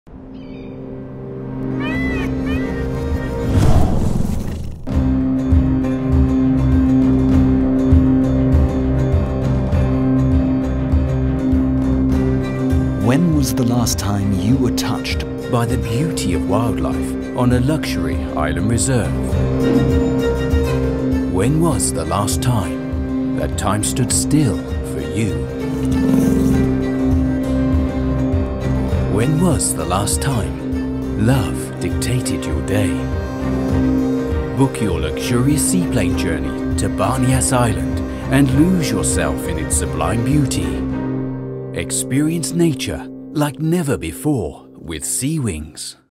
Seawings Seaplane | Experience Sir Bani Yas Island
Take a private seaplane flight filled with exquisite views of the shimmering coastline to Sir Bani Yas Island with Seawings. A short flight from Abu Dhabi, this luxury island reserve is a perfect a weekend getaway and if you are short on time, ideally even for a daytrip with Seawings. Experience the well known Sir Bani Yas Island safari followed by a delicious meal at Anantara and much more.